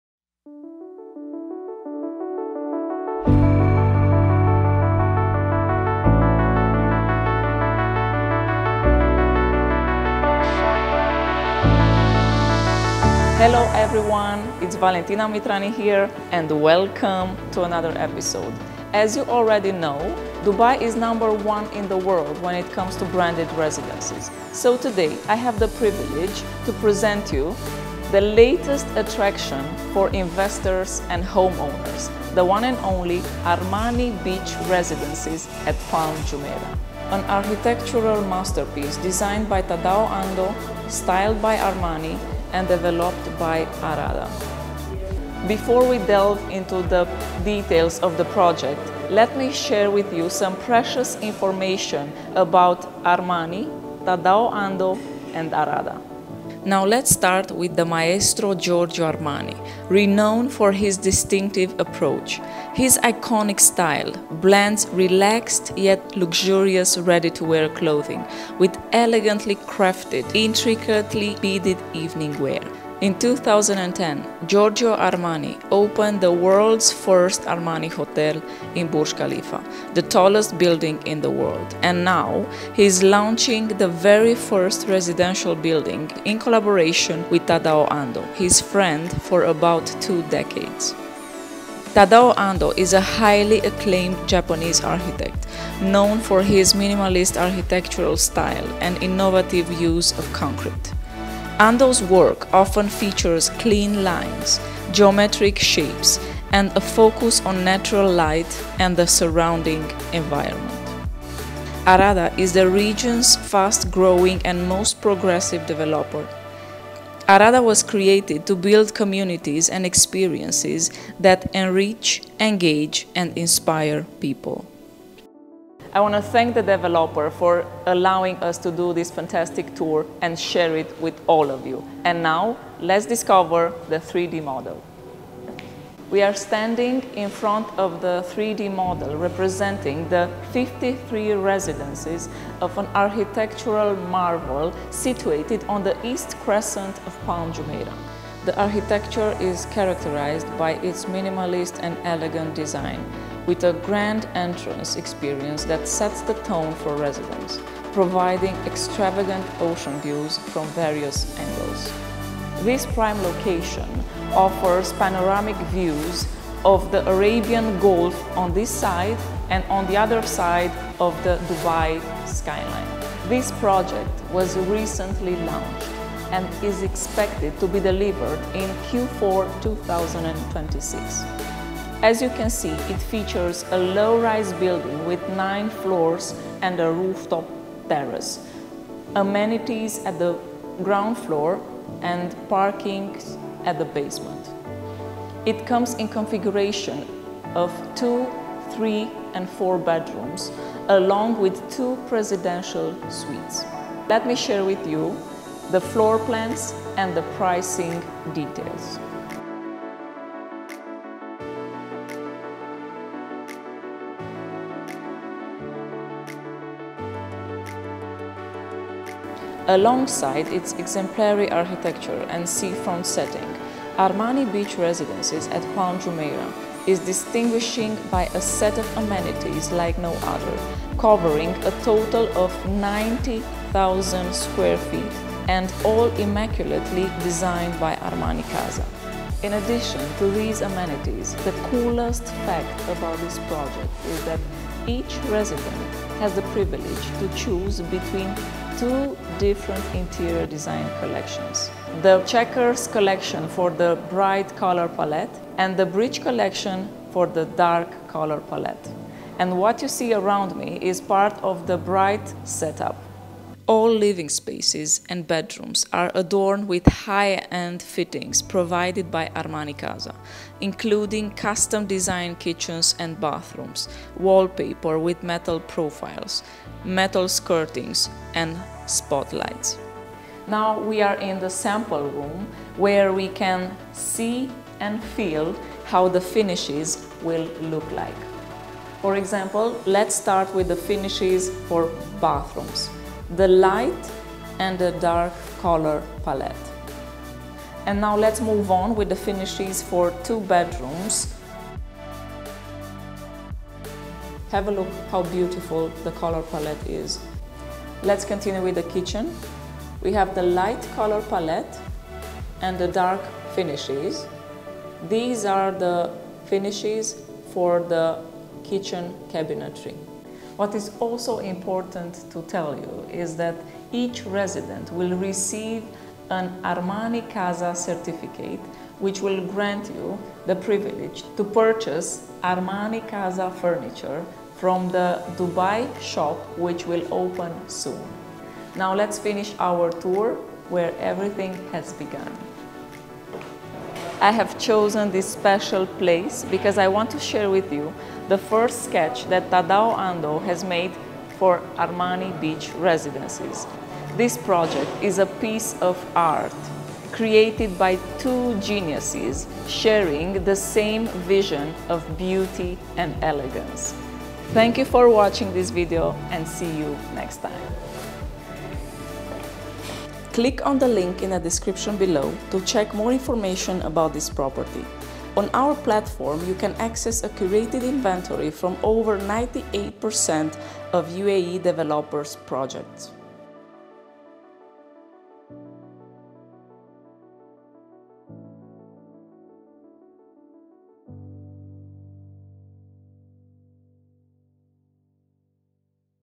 Discover Armani Beach Residences at Palm Jumeirah Dubai | Giorgio Armani | Tadao Ando | Arada
If you want to purchase a property in Dubai, please get in touch.

Discover the Epitome of Elegance at Armani Beach Residences, Palm Jumeirah

Valentina Mitrani  unveils the unparalleled luxury of the One and Only Armani Beach Residences, nestled on the pristine East Crescent of Palm Jumeirah. This architectural marvel, a symphony of design by Tadao Ando and styled by Armani, offers an exclusive sanctuary for the discerning.

Key Highlights:

Dive into the world of branded residences with this new gem in Dubai, where luxury meets iconic design.
Get to know the visionaries behind this project: the legendary Giorgio Armani, renowned for his elegant fashion and lifestyle creations, and Tadao Ando, a master of minimalist architecture.
Arada, the developer, brings this vision to life, promising an unparalleled living experience in one of Dubai's most sought-after locations.
Project Insights:

The Armani Beach Residences boast 53 exclusive units, offering breathtaking views of the Arabian Gulf and the Dubai skyline.
Scheduled for completion in Q4 2026, the residences range from 2 to 5-bedroom configurations, with prices starting from 21 million AED.
Owners will have the privilege to adorn their homes with pieces from ARMANI CASA, soon to open in Dubai, with each residence coming with an Armani/Casa Certificate.
Amenities Beyond Compare:

Over 90,000 square feet of Armani/Casa designed amenities, including a residents-only spa, cigar lounge, movie theatre, children’s playroom, and a private beach.
Choose from two exclusive interior design collections by Armani/Casa: the light Checkers collection or the dark Bridge collection.
Exclusive Tour:

Valentina takes you through the project's 3D model, showcasing its elegant design and prime location.
Explore the beautifully designed sample room, featuring the Checkers collection's light palette, offering a glimpse into the refined lifestyle awaiting residents.
A Confluence of Geniuses:

Discover the first sketches by Tadao Ando and Giorgio Armani for this project, a testament to their shared vision of beauty and elegance.
Join us in this exquisite journey through one of Dubai's most anticipated projects, where every detail is a masterpiece of design and luxury.